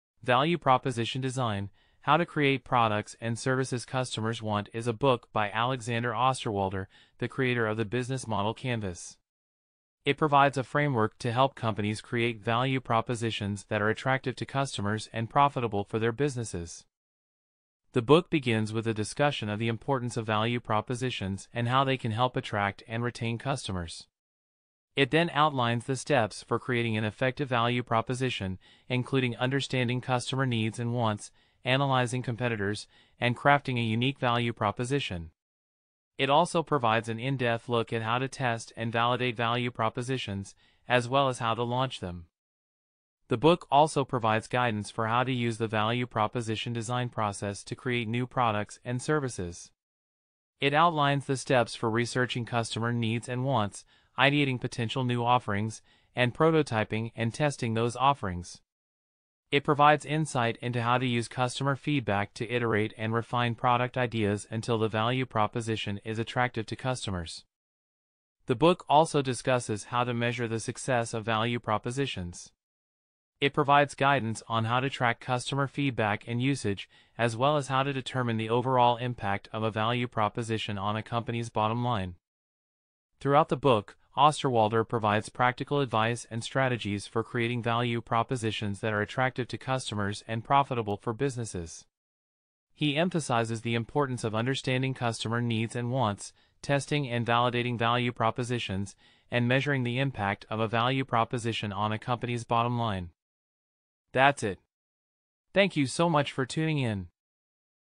Value Proposition Design by Alexander Osterwalder
In this video, I'm giving you a 2 minute summary of the book Value Proposition Design by Alexander Osterwalder.

Alexander Osterwalder is a bestselling author and a leading business strategist. His book Value Proposition Design is all about how to create products and services that customers want. Osterwalder explains the four steps to creating value for customers: identify customer jobs, pains and gains; design products and services; test and deliver value propositions; and finally, analyse customer feedback. He also provides real-life examples from successful companies such as Apple and Airbnb to illustrate how to execute the steps.

So if you're looking for a concise and informative guide on how to create value propositions that your customers will love, this book is definitely worth checking out.